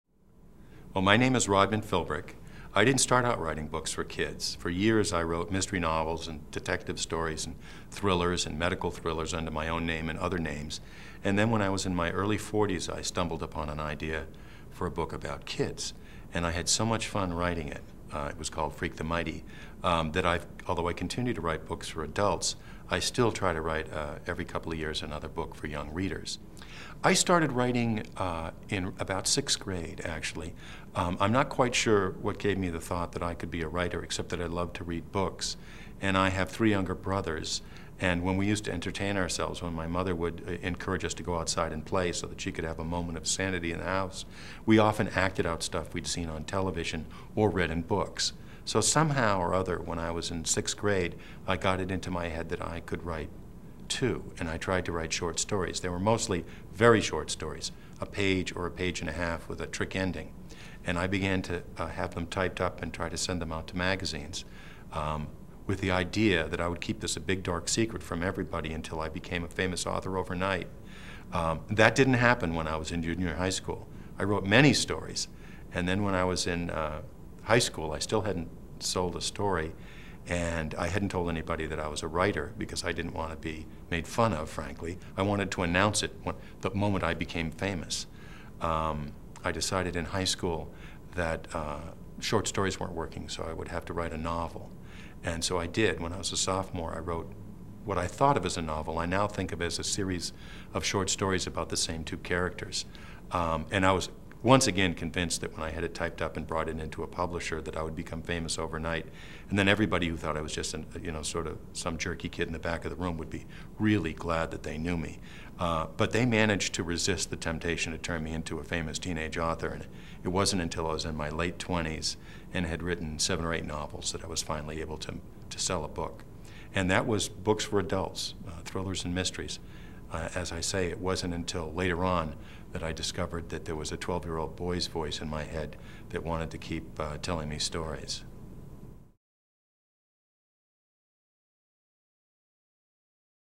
Meet Rodman Philbrick
Philbrick began writing stories in elementary school, but kept it a secret because he didn't want to be teased. After a long career of writing mysteries and thrillers for adults, he began writing books for younger readers.